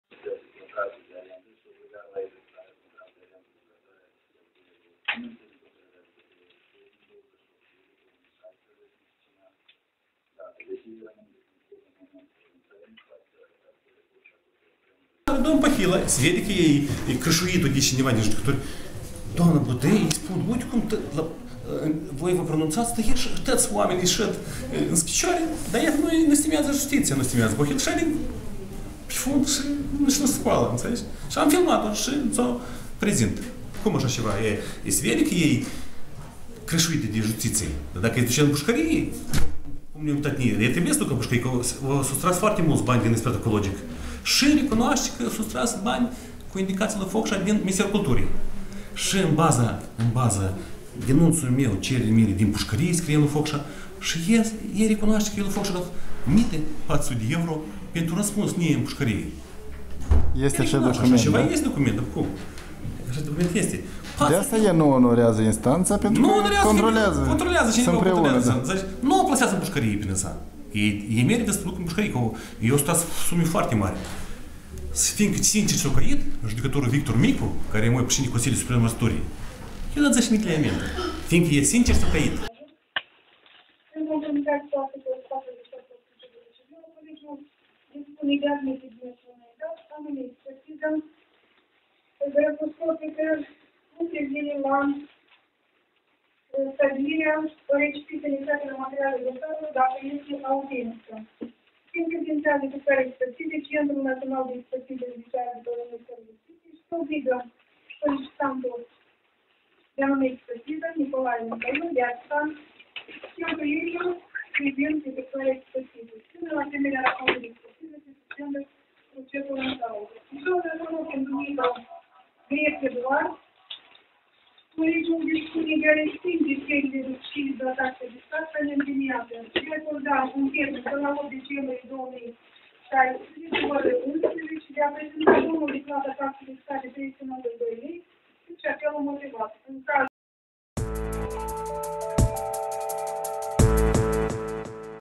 Zice că Pohilă n-a onorat instanța în mod sfidător - Curaj.TV
Oleg Chirvas spune că Nadejda Pohilă, fostă contabilă la Ministerul Culturii, prezentă la o ședință de judecată, a sfidat instanța și nu s-a ridicat cînd se pronunțau deciziile și încheierile, manifestînd astfel lipsă de respect.